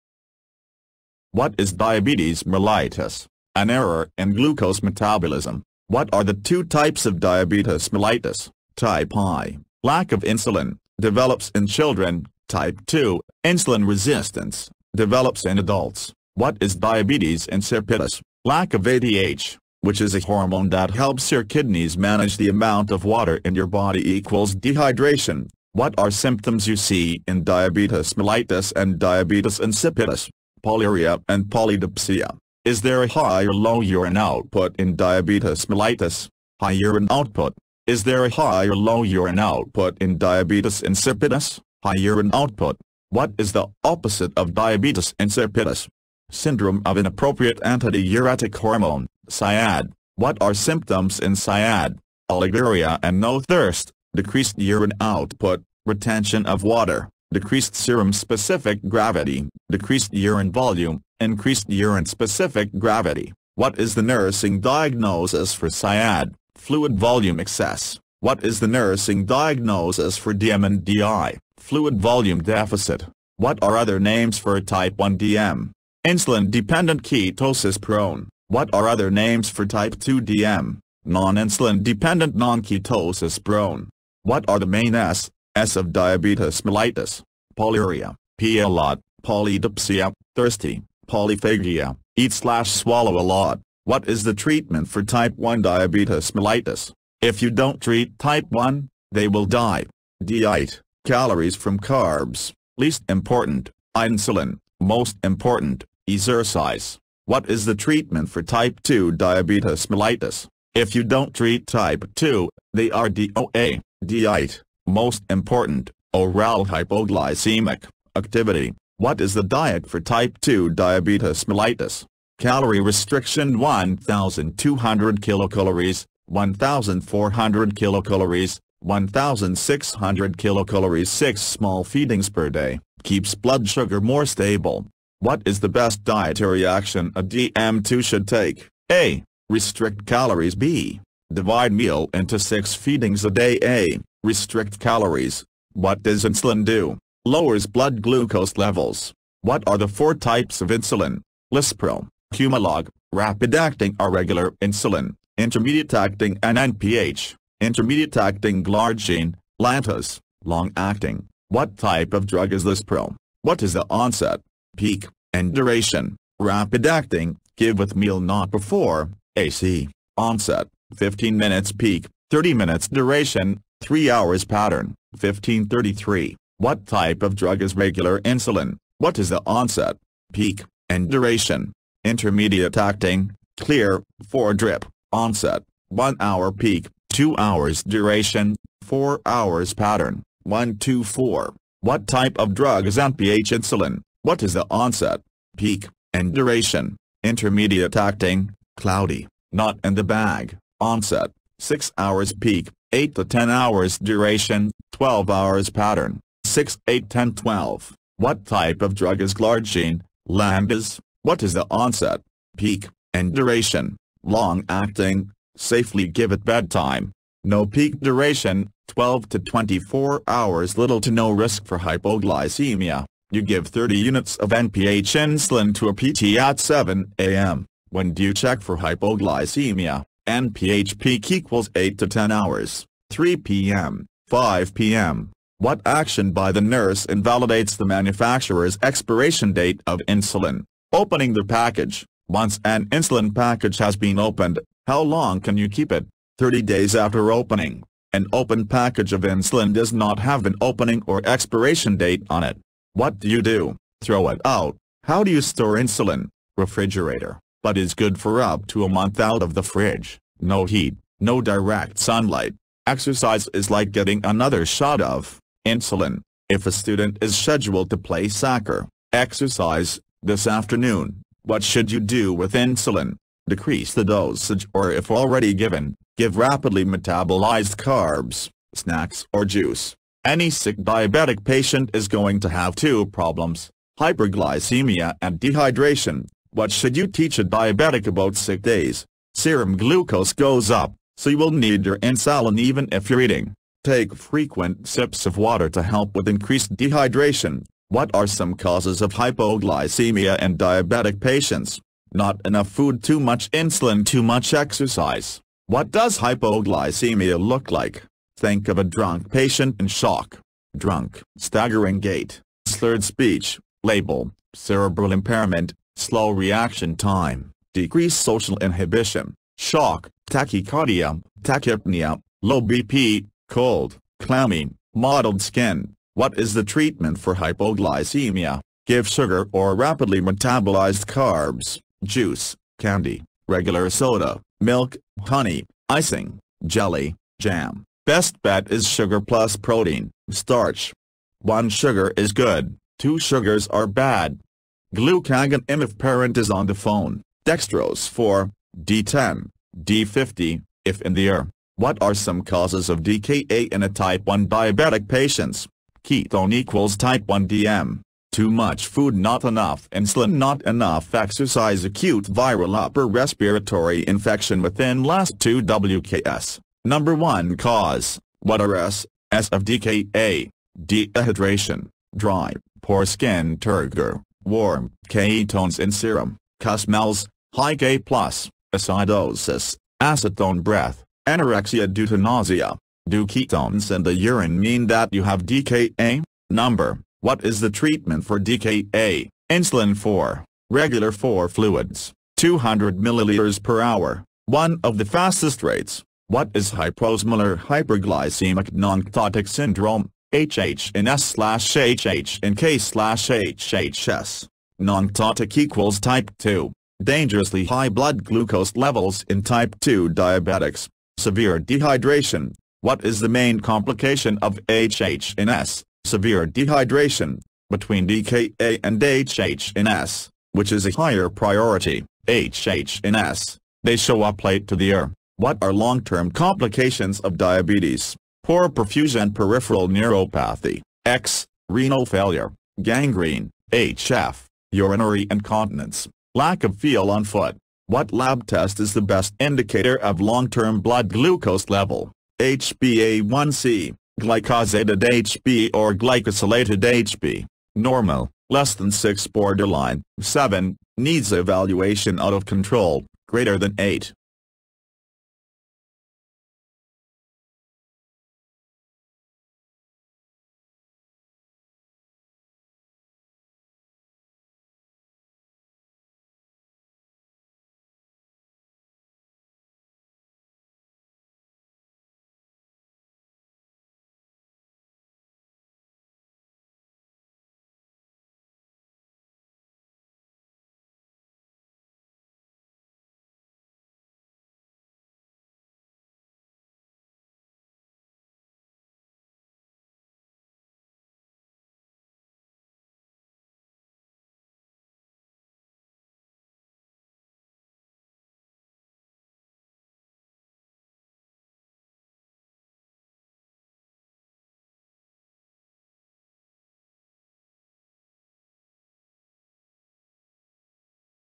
NCLEX; Mark Klimek Lecture 5 DM, DI, & Insulin
What is diabetes mellitus?
An error in glucose metabolism.

What are the 2 types of diabetes mellitus?
Type I (Lack of insulin, develops in children)
Type II (insulin resistance, develops in adults)

What is diabetes insipidus?
Lack of ADH, which is a hormone that helps your kidneys manage the amount of water in your body = Dehydration

What are symptoms you see in diabetes mellitus AND diabetes insipidus?
Polyuria and polydipsia

Is there a high or low urine output in diabetes mellitus?
High urine output

Is there a high or low urine output in diabetes insipidus?
High urine output

What is the opposite of diabetes insipidus?
Syndrome of Inappropriate Antidiuretic Hormone (SIADH)

What are symptoms in SIADH?
- Oliguria and no thirst
- Decreased urine output
- Retention of water (decreased SERUM specific gravity)
- Decreased urine volume (increased URINE specific gravity)

What is the nursing diagnosis for SIADH?
Fluid volume excess

What is the nursing diagnosis for DM and DI?
Fluid volume deficit

What are other names for Type 1 DM?
Insulin dependent
Ketosis prone

What are other names for Type 2 DM?
Non-insulin dependent
Non-ketosis prone

What are the main s/s of diabetes mellitus?
Polyuria (Pee a lot)
Polydipsia (Thirsty)
Polyphagia (Eat/swallow a lot)

What is the treatment for Type 1 diabetes mellitus?
"If you don't treat Type 1, they will DIE."

(D)iet (calories from carbs, least important)
(I)nsulin (most important)
(E)xercise

What is the treatment for Type 2 diabetes mellitus?
"If you don't treat Type 2, they are DOA."

(D)iet (Most important)
(O)ral hypoglycemic
(A)ctivity

What is the diet for Type 2 diabetes mellitus?
Calorie restriction
1200 kcal, 1400 kcal, 1600 kcal
6 small feedings per day (keeps blood sugar more stable)

What is the best dietary action a DM2 should take?

a. Restrict calories
b. Divide meal into 6 feedings a day
a. Restrict calories

What does insulin do?
lowers blood glucose levels

What are the 4 types of insulin?
Lispro (Humalog): Rapid-acting
R-Regular insulin: Intermediate-acting
N-NPH: Intermediate-acting
Glargine (Lantus): Long-acting

What type of drug is Lispro? What is the onset, peak, and duration?
Rapid-acting, give WITH meal not before (AC)

Onset: 15 min
Peak: 30 min
Duration: 3 hrs
Pattern: 15-30-3

What type of drug is regular insulin? What is the onset, peak, and duration?
Intermediate-acting, CLEAR, IV drip

Onset: 1 hr
Peak: 2 hrs
Duration: 4 hrs
Pattern: 1-2-4

What type of drug is NPH insulin? What is the onset, peak, and duration?
Intermediate-acting, CLOUDY, not in the bag.

Onset: 6 hrs
Peak: 8-10 hrs
Duration: 12 hrs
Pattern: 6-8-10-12

What type of drug is Glargine (Lantus)? What is the onset, peak, and duration?
Long-acting, safely give at bedtime

NO PEAK
Duration: 12-24 hrs
Little to no risk for hypoglycemia

You give 30 units of NPH insulin to a pt at 7 a.m. When do you check for hypoglycemia?
NPH Peak = 8-10hrs

3pm - 5pm

What action by the nurse invalidates the manufacturer's expiration date of insulin?
Opening the package

Once an insulin package has been opened, how long can you keep it?
30 days after opening

An open package of insulin does not have an opening or expiration date on it. What do you do?
Throw it out.

How do you store insulin?
Refrigerator, but is good for up to a month out of the fridge, no heat, no direct sunlight.

Exercise is like getting another shot of...
insulin.

If a student is schedule to play soccer (exercise) this afternoon, what should you do with insulin?
Decrease the dosage OR if already given, give rapidly metabolized carbs (snacks or juice).

Any SICK diabetic patient is going to have two problems:
Hyperglycemia and dehydration

What should you teach a diabetic about sick days?
Serum glucose goes up, so you will need your insulin even if you're eating. Take frequent sips of water to help with increased dehydration.

What are some causes of hypoglycemia in diabetic patients?
Not enough food
Too much insulin
Too much exercise

What does hypoglycemia look like?
Think of a drunk patient in shock.

DRUNK:
- Staggering gait
- Slurred speech
- Labile (Cerebral impairment)
- Slow reaction time
- Decrease social inhibition

SHOCK:
- Tachycardia, tachypnea, low BP
- Cold/clammy, mottled skin

What is the treatment for hypoglycemia?
- Give SUGAR or rapidly metabolized carbs (juice, candy, regular soda, milk, honey, icing, jelly, jam) ** Best bet is sugar + protein/starch. 1 sugar is good, 2 sugars are bad.

- Glucagon IM if parent is on the phone
- Dextrose IV (D10, D50) if in the ER

What are some causes of DKA in a Type 1 diabetic patients?
Ketone = Type 1 DM